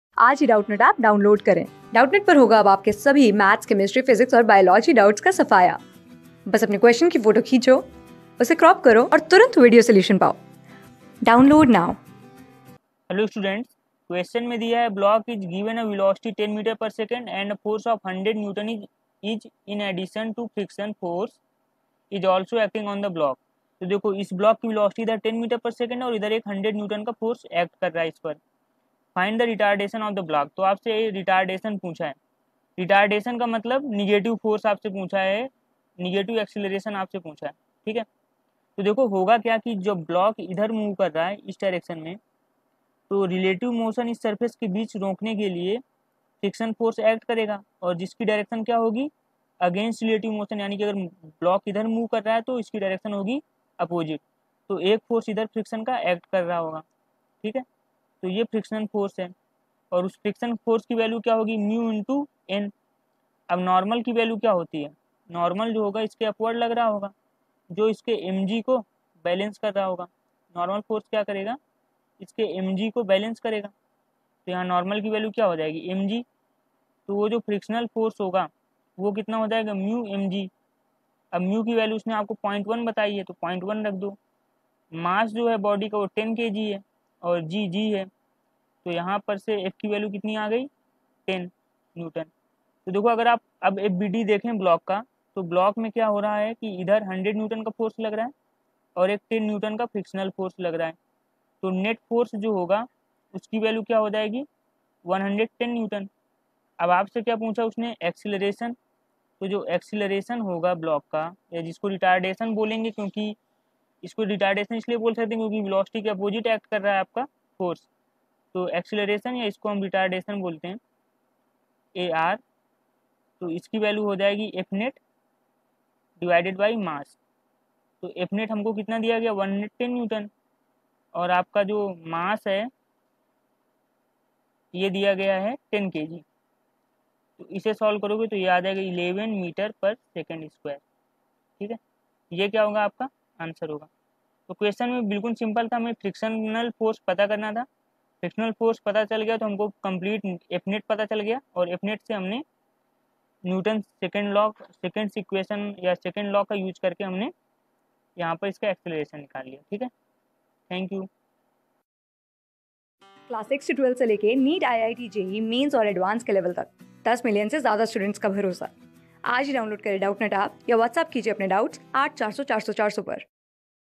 A block is given a velocity of 10 m/s and a force of 100 N in addition to friction force is also...
A block is given a velocity of 10 m/s and a force of 100 N in addition to friction force is also acting on the block. Find the retardation of the block?
Class: 12
Subject: PHYSICS
Chapter: NLM & FRICTION
Board:IIT JEE

👉 Have Any Query? Ask Us.